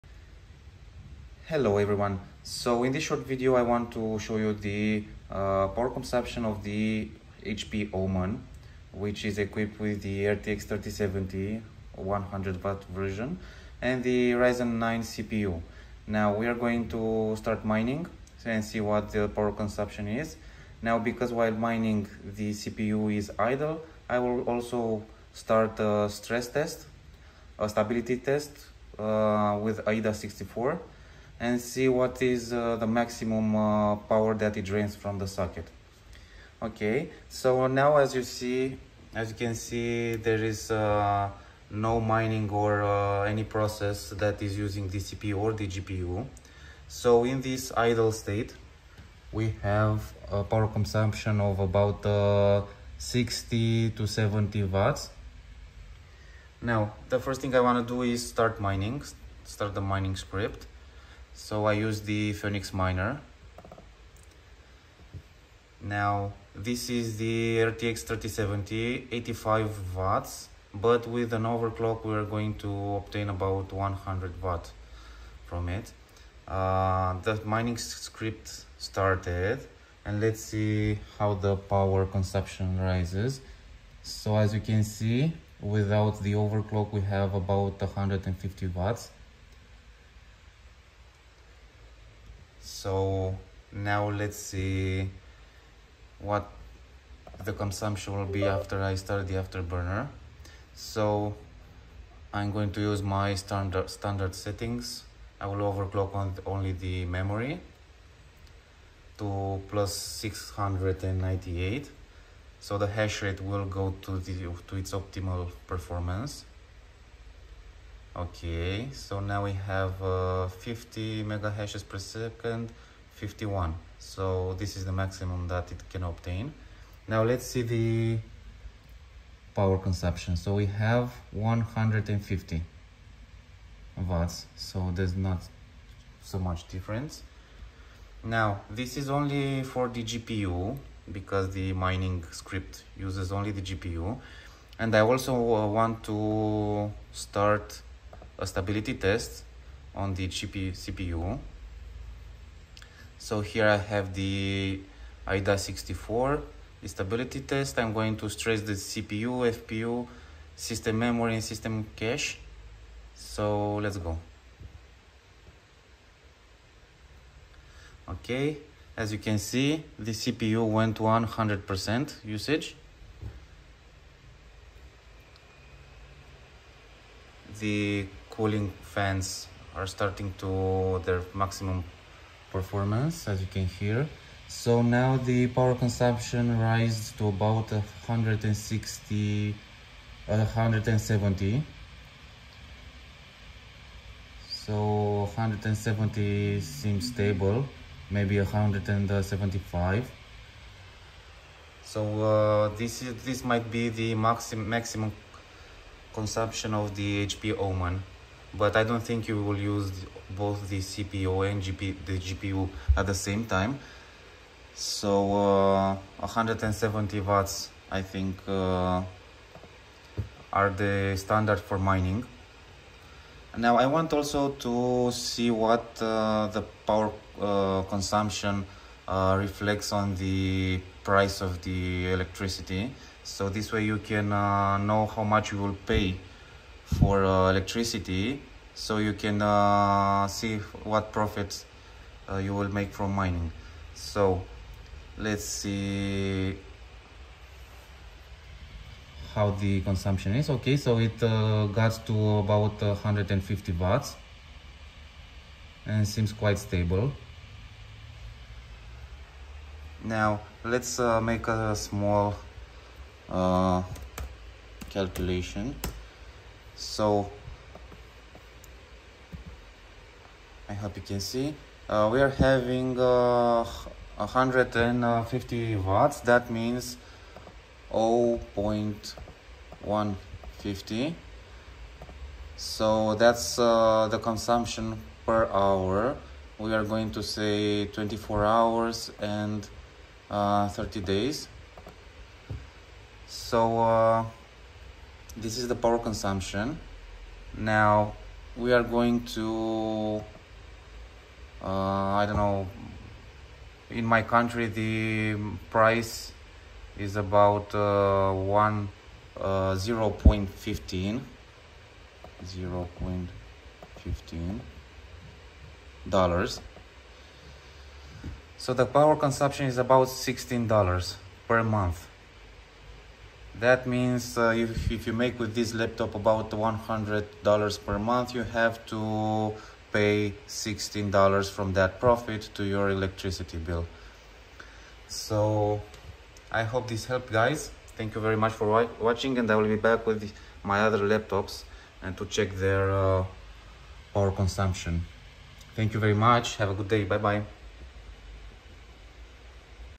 HP OMEN Power Consumption in Full Load - RTX 3070 Costs & Profitability
Here you can see the power consumption of the HP OMEN in idle mode, GPU full load while mining, CPU full load in a stability test and also a short calculation of the profitability for mining crypto after paying the power consumption.

Full Laptop Specifications:

HP OMEN 15-en1003nq Gaming Laptop
CPU:  AMD Ryzen™ 9 5900HX
Display: 15.6", Full HD
Memory: 16Gb RAM
Storage: 512Gb SSD
Dedicated GPU: Nvidia RTX 3070 100W (overcloaked)
Price:  1600$

The video has been filmed using the Iphone 13 PRO + DJI Osmo Mobile 5

Please check my other videos with reviews on laptops, mining, settings and many more.